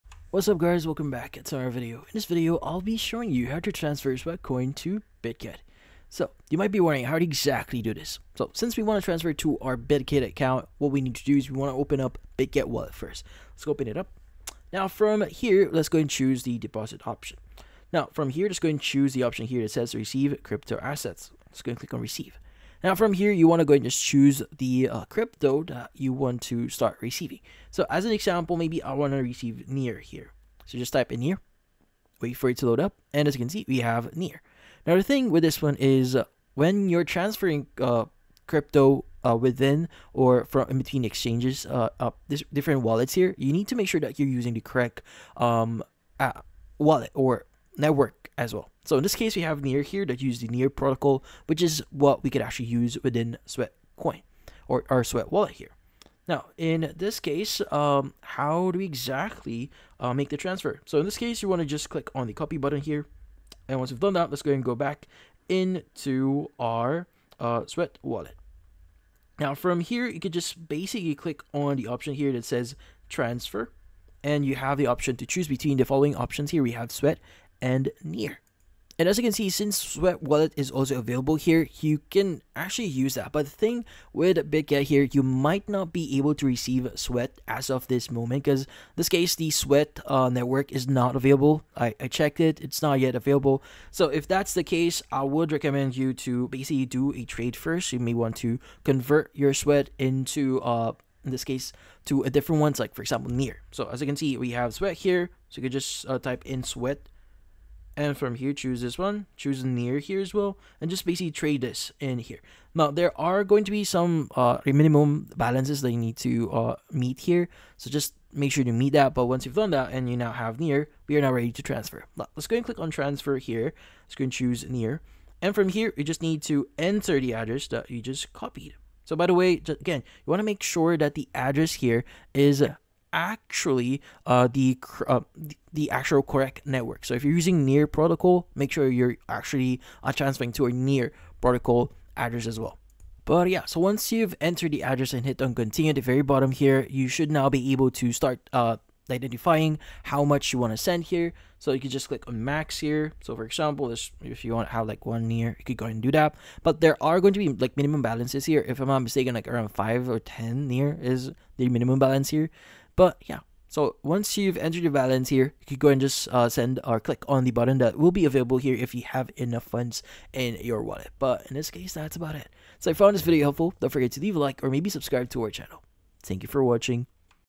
How To Transfer/ Send Sweatcoin To Bitget- Step By Step
How do I send sweatcoin to bitget? In this video tutorial, I’ll show you howto transfer or move sweatcoin to bitget. Enjoy!

💸first month for $1